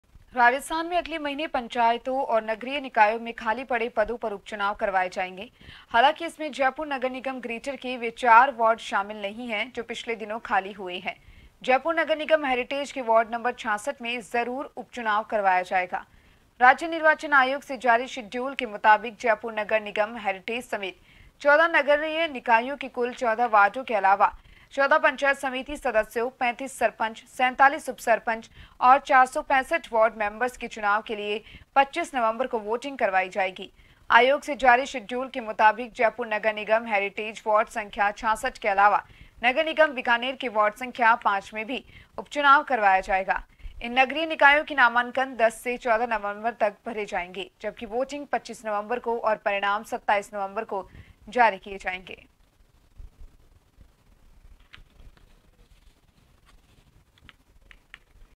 राजस्थान मे निकाय-पंचायतो मे उपचुनाव की घोषणा, 35 सरपंच, 465 पंचो के चुनाव के लिये मतदान 25 नवंबर को
NEWS CREDIT-GS